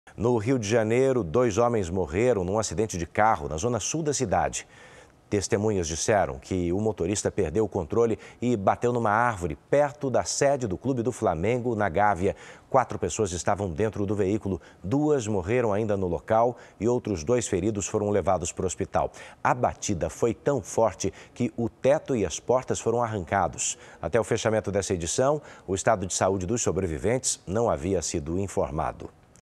Acidente de carro na zona sul do Rio de Janeiro deixa dois mortos
Na zona sul do Rio de Janeiro, dois homens morreram em um acidente de carro. Testemunhas relataram que o motorista perdeu o controle e bateu contra uma árvore na Gávea, região próxima à sede do clube do Flamengo. Quatro pessoas estavam dentro do veículo. Duas pessoas faleceram no local e outras duas foram levadas feridas para o hospital. A força da batida foi suficiente para arrancar o teto e as portas do automóvel. Até o fechamento desta edição, não havia informações sobre o estado de saúde dos sobreviventes.